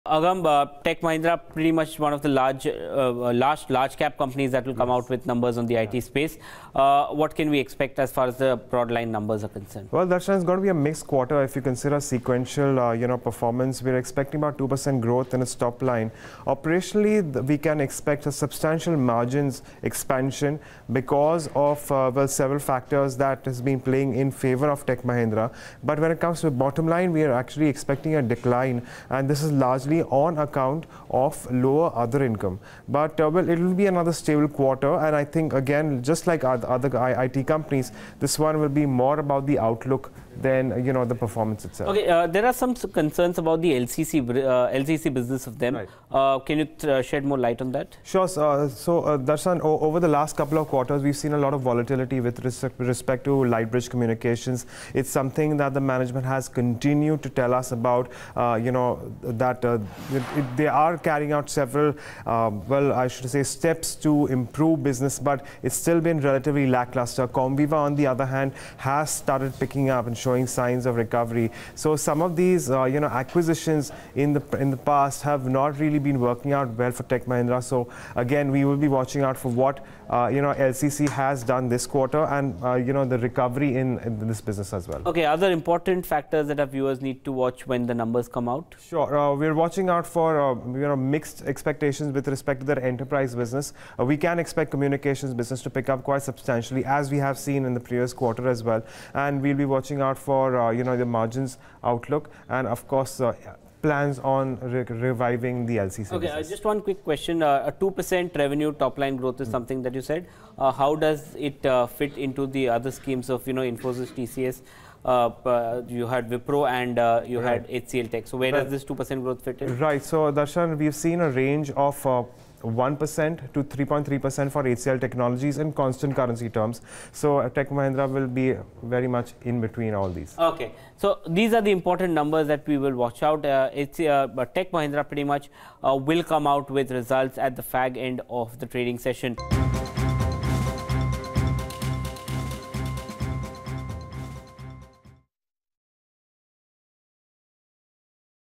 Q3 With BQ: What To Expect From Tech Mahindra This Quarter?
Tech Mahindra's Q3 expectations.